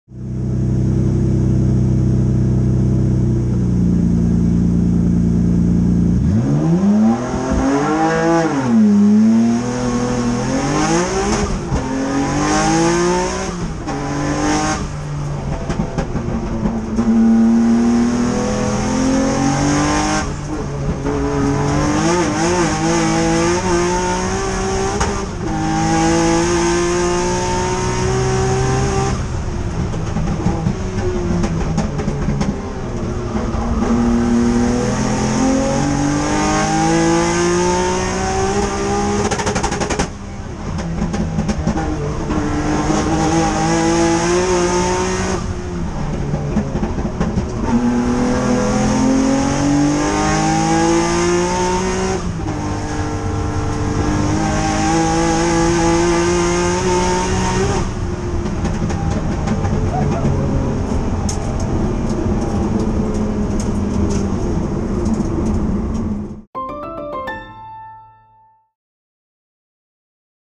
Onboard bonkers 1,000PS Skyline R32's Goodwood shootout run
Jake Hill was a star at Goodwood SpeedWeek presented by Mastercard, taking the crazed Calsonic Nissan Skyline R32 round the Goodwood Motor Circuit.

He was back at it again at the Festival of Speed, this time in an HKS-liveried Skyline, running in the Timed Shootout and winning the touring car class. This was his shootout final run.